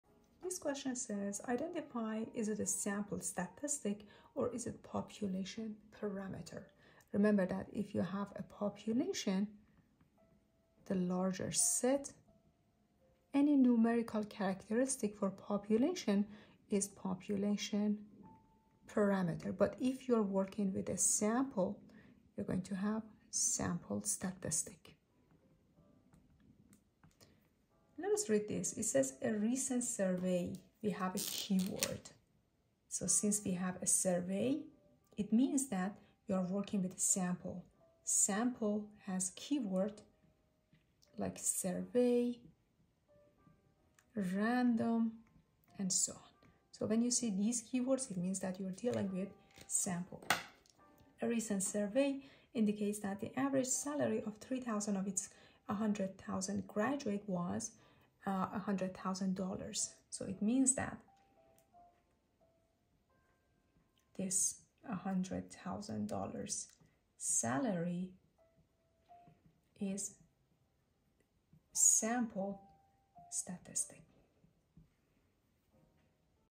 Statistics: Sample Statistic or Population Parameter?
Determine whether the numerical value is a parameter or a statistic.

 A recent survey by the alumni of a major university indicated that the average salary of 7500 of its 200,000 graduates was $95,000.